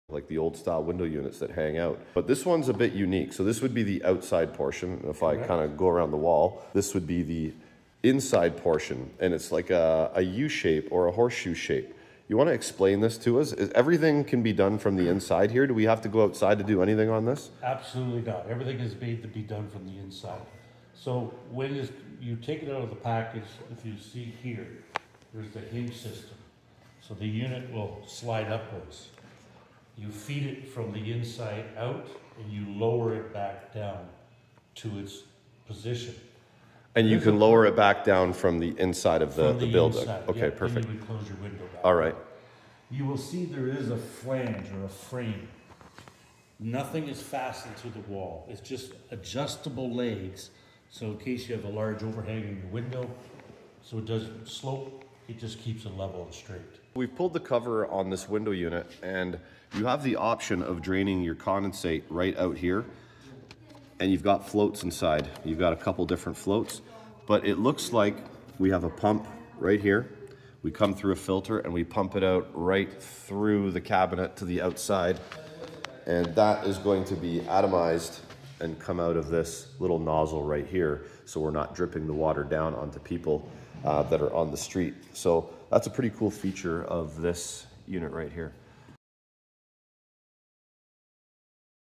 Midea Package Window Heat Pump
Packaged Window Heat Pump...

Midea RAC designed this Window Package Heat Pump for high rise buildings in New York City.

Word on the street is tenant spaces in some areas will have a max temp they can be at, just like they have a min temp they must maintain.  Essentially, some rented spaces will be forced to provide air conditioning if they don't already.

I think the atmomized condensate is a cool feature.